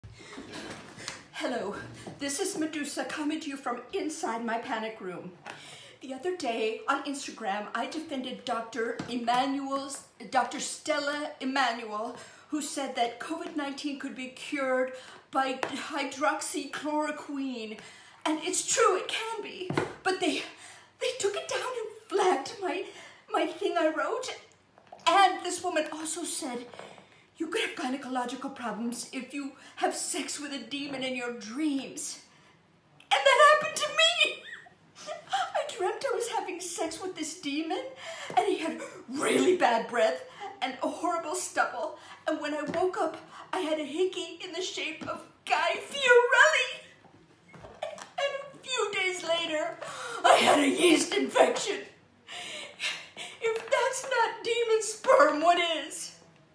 Medusa defends Demon Sperm Doctor! IMG 9165
Julie Brown as Medusa/Madonna comes out for Dr. who has cure for Covid-19 and warns against Demon Sperm! Medusa madonna Covid-19